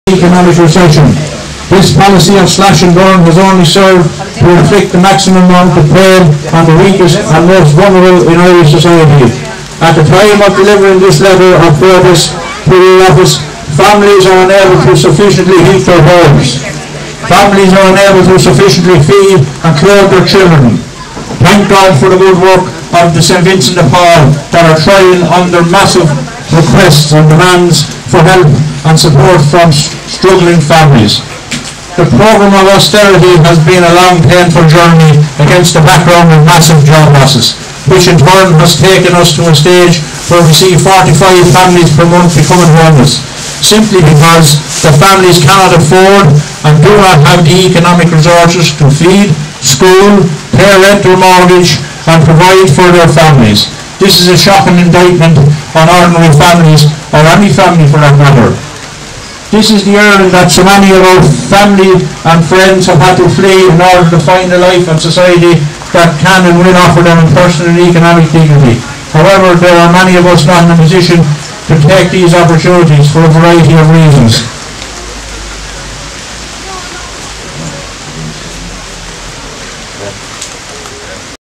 Athy Water Protest Nov. 1st attended by over 1000
On a very rainy 1st of November over 1000 Athy people  turned  out for a Protest march from the Arch Centre to Emily Square where a rally was held and adressed by a number  of non Political speakers.